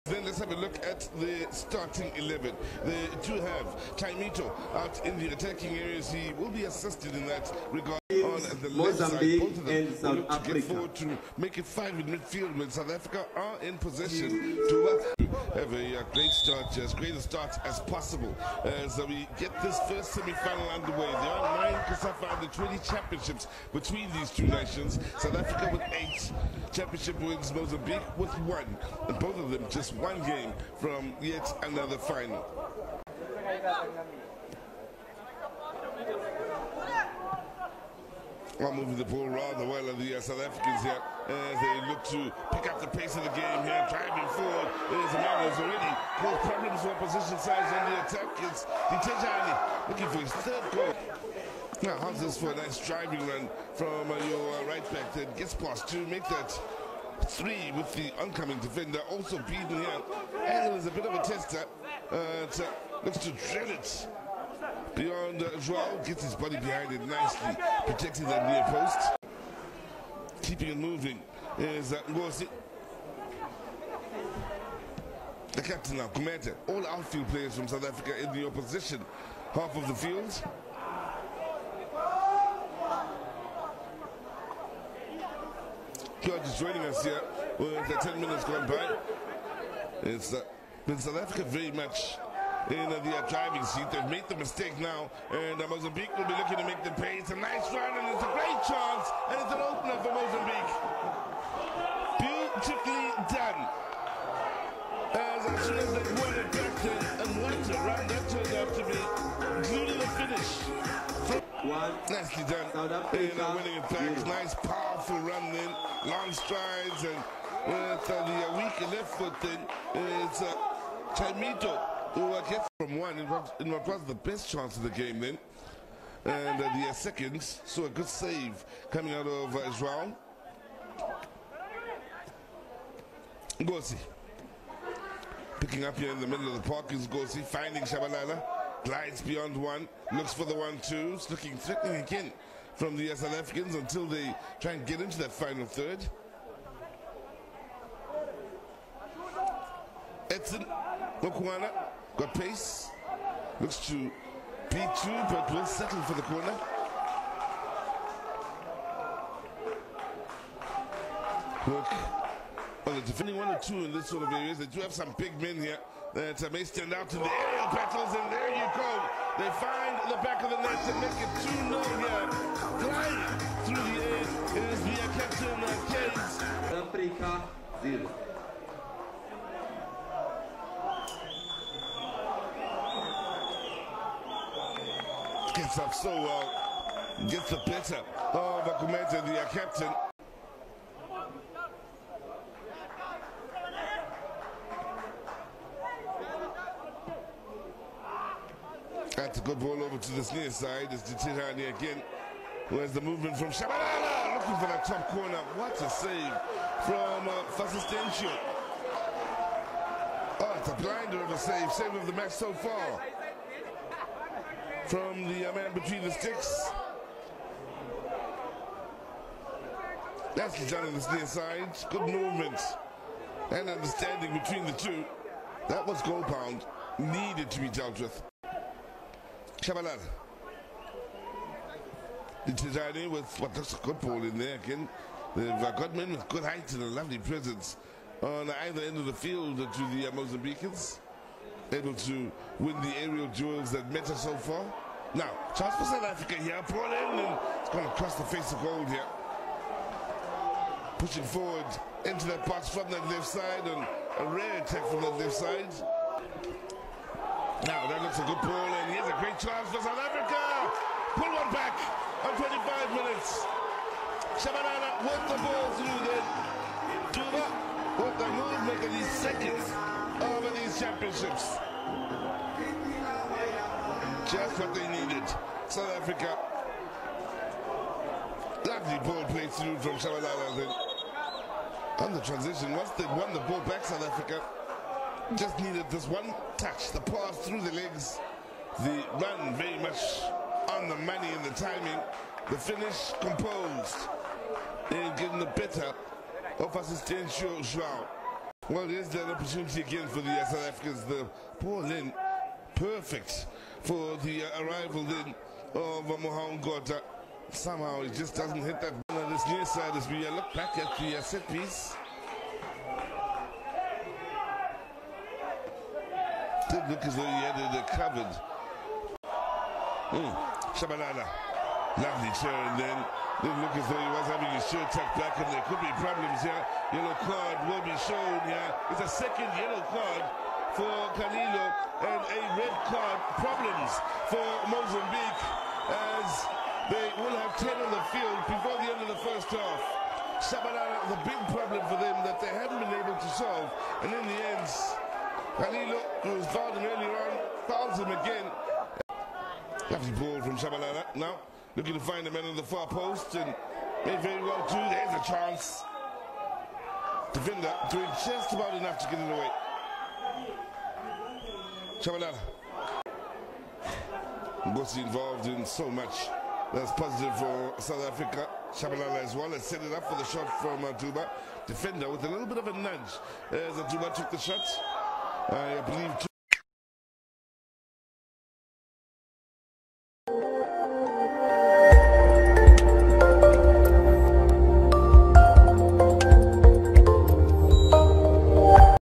Mozambique Vs South Africa (2 - 1) All Goals & Extended Highlights – COSAFA MEN'S UNDER-20 - Semis
young Mambas of Mozambique advanced on the strength of an excellent start at the Somhlolo National Stadium in Lobamba, defeating South Africa 2-1. Chaimito Alfandega danced past a couple of Amajita defenders before tapping his finish past onrushing goalkeeper Aden Dreyer to put Mozambique into the lead in the 11th minute. Only four minutes later the South African defense was caught sleeping again when Mozambique captain Keyns Abdala towered above his marker to nod in a corner kick, making it 2-0 at the end of the opening quarter hour.

South Africa fought back but will be disappointed they were not able to complete a comeback. In the 25th minute the Mozambique lead was cut in half when a turnover led to Mduduzi Shabalala’s brilliant through ball finding Brenden Duba, who slipped his low finish past keeper João Fasistencio. It appeared that South Africa would surge back when Mozambique’s Calilo Traore was sent off just before halftime for a second yellow card but Amajita could not muster up an equalizer with their day being summed up by a stunning save from point blank range by Fasistencio in the 78th minute.